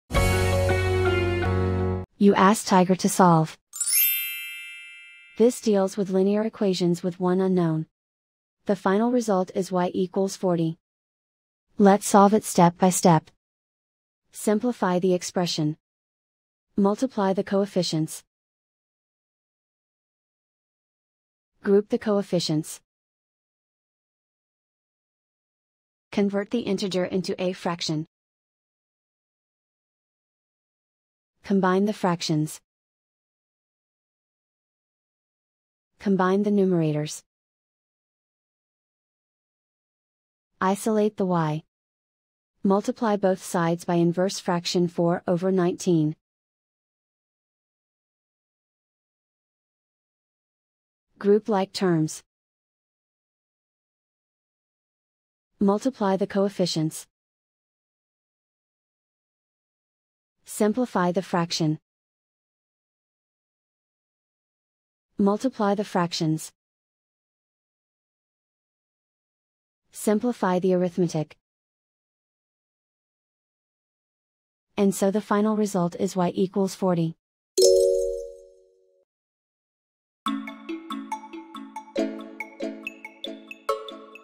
Solve 2(y)+2(11/8y)=190: Linear Equation Video Solution | Tiger Algebra
Struggling with 2(y)+2(11/8y)=190? Watch our step-by-step video on Tiger Algebra to master this linear equation effortlessly.